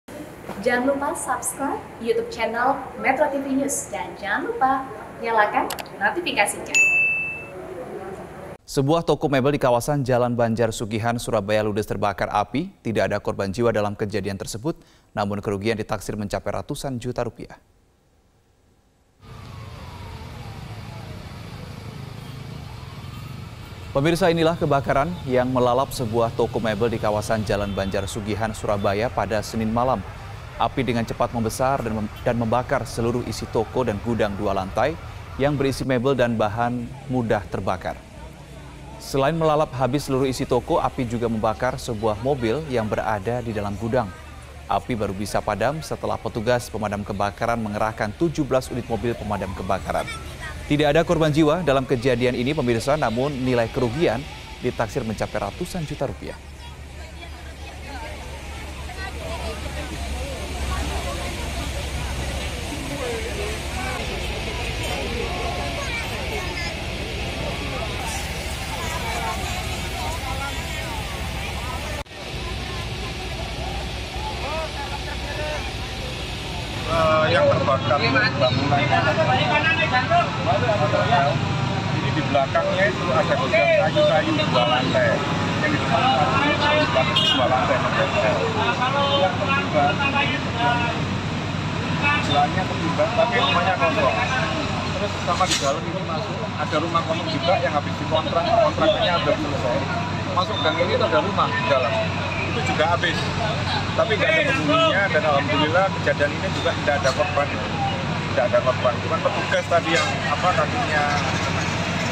Gudang Mebel di Surabaya Terbakar
Sebuah toko mebel di kawasan Jalan Banjar Sugihan, Surabaya, ludes terbakar api pada Senin (12/9/2022) malam. Tidak ada korban jiwa dalam kejadian ini, namun kerugian ditaksir mencapai ratusan juta rupiah.

Api dengan cepat membesar dan membakar seluruh isi toko dan gudang dua lantai yang berisi mebel dan bahan mudah terbakar. Api juga membakar sebuah mobil yang berada di dalam gudang. Api baru bisa padam setelah petugas pemadam kebebakaran mengerahkan 17 unit mobil pemadam.